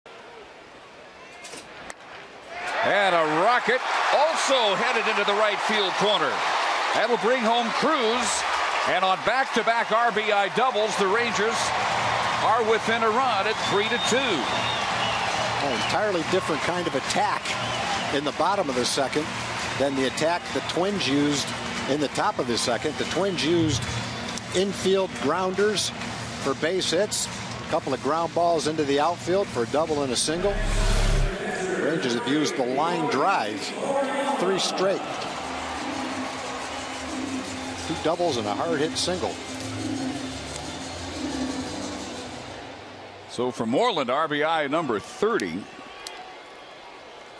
2011/07/26 Moreland's RBI double
MIN@TEX: Moreland rips an RBI double in the second